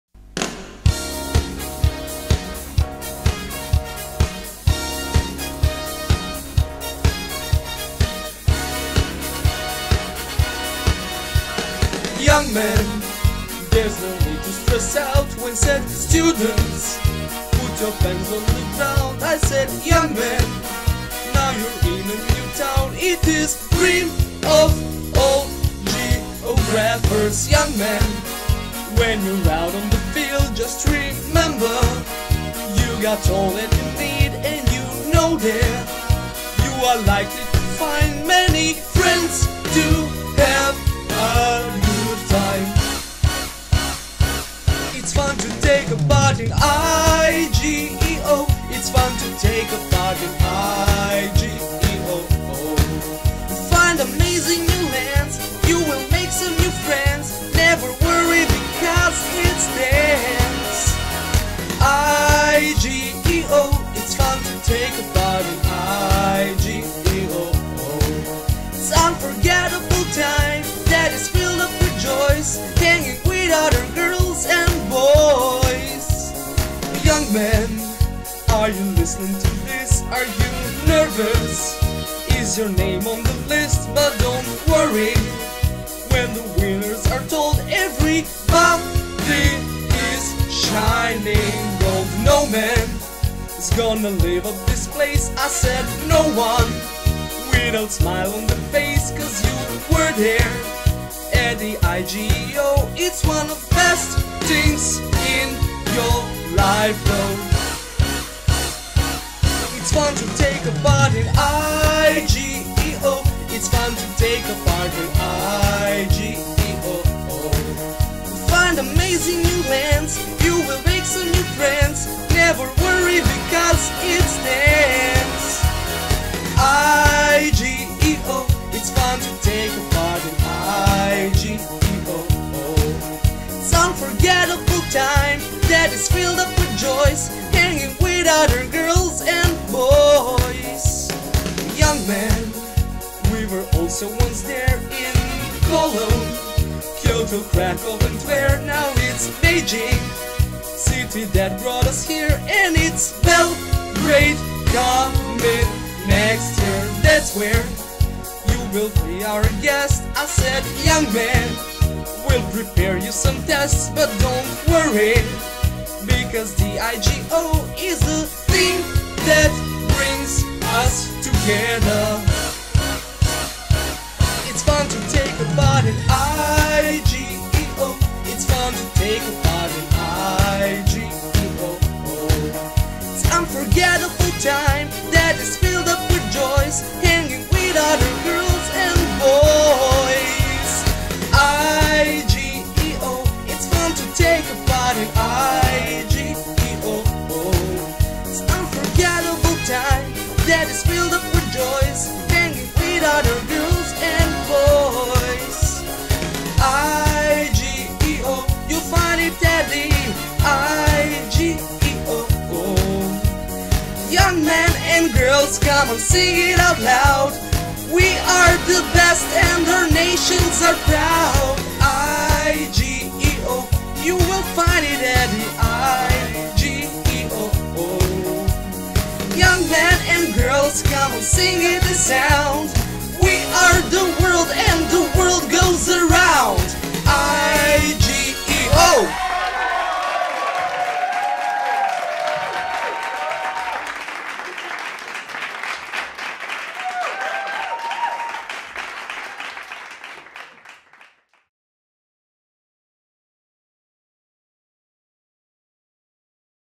I.G.E.O. Serbia Team 2016.
Emilija Đokanović, Marko Todorović, Đorđe Petković i Mladen Stanković su najbolji srednjoškolaci iz geografije u Srbiji. Oni čine naš geografski olimpijski tim koji će od 16-22 avgusta u Pekingu branti boje naše zemlje i pokušati da nadmaše uspeh njihovih drugara iz prethodnih generacija. Promo spot sa kojim će se predstaviti u revijalnom delu takmičenja je proizvod zajedničkog rada Regionalnog centra za talente Beograd II, čiji su oni članovi, i naše televizije koja će ih i dalje pratiti i podržavati. Značajna vest je da je Beograd sledeće godine domaćin 14. po redu Internacionalne geografske olimpijade i da ćemo imati priliku da ugostimo preko trista najboljih đaka Sveta. Poželimo sreću našim genijalcima i uživajmo u njihovoj pesmi!